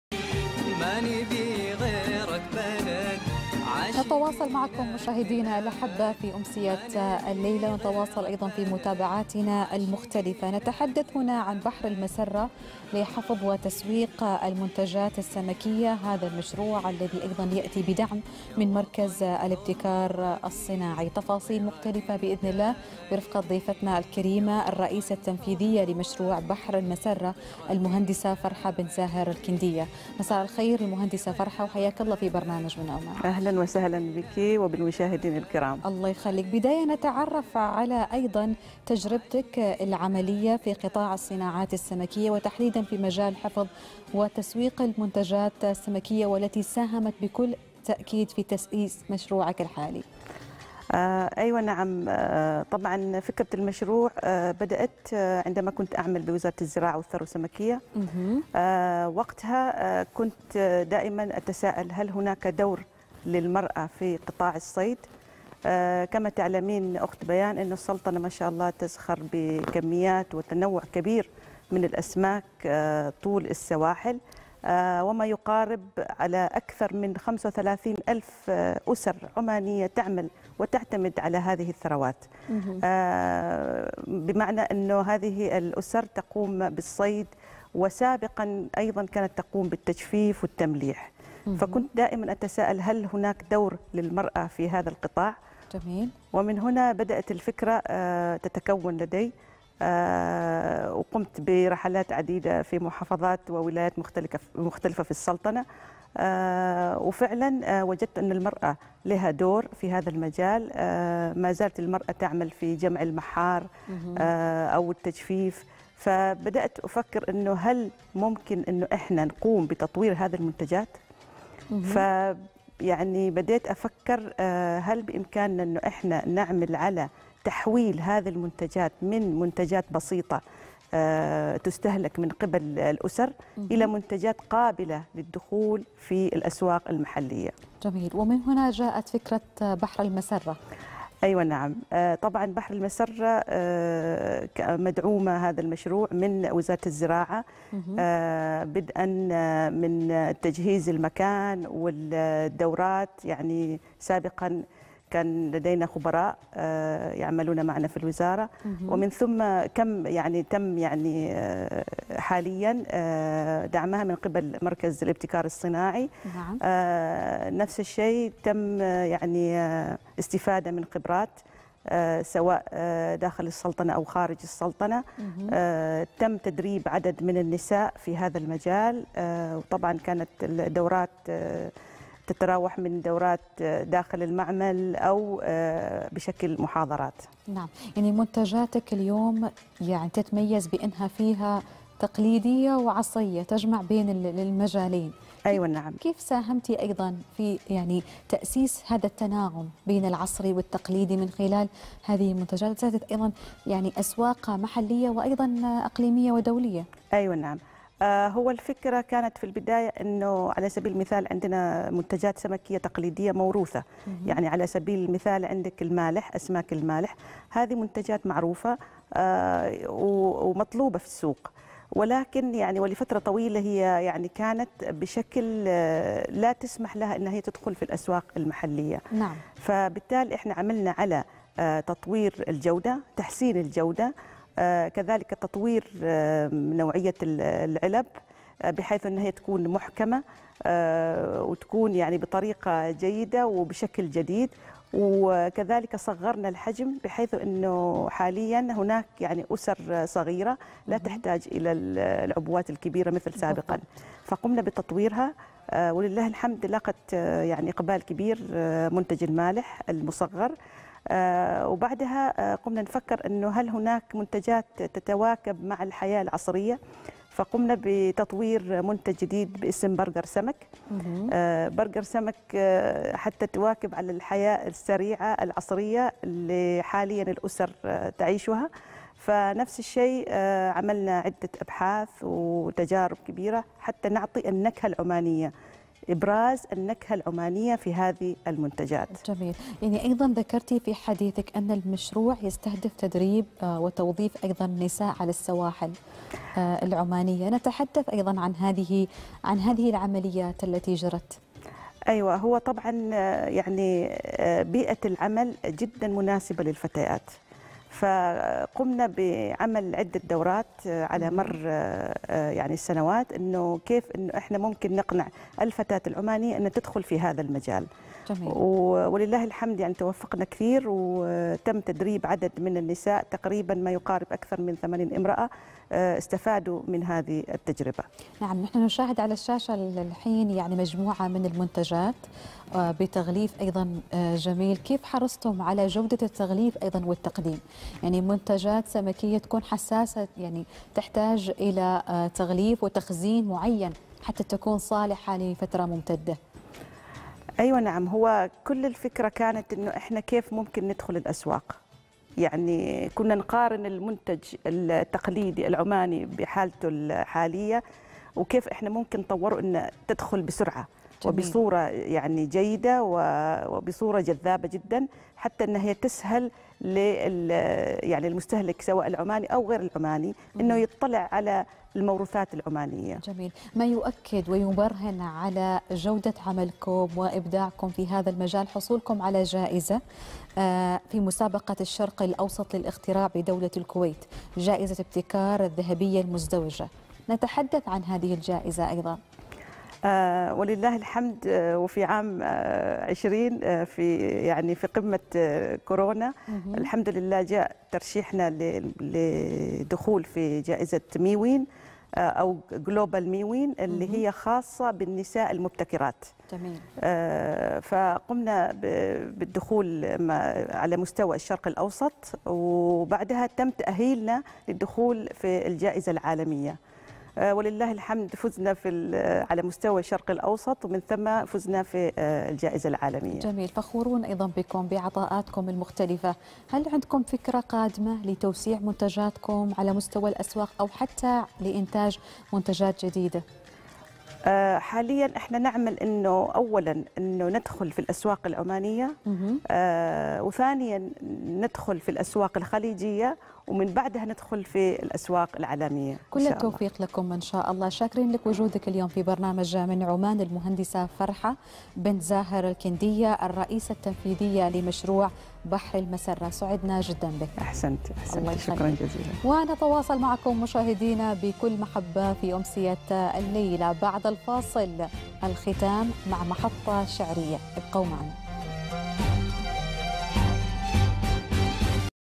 بحر المسرة لحفظ وتسويق المنتجات السمكية
المهندسة فرحة بنت زاهر الكندية - الرئيسة التنفيذية لمشروع بحر المسرة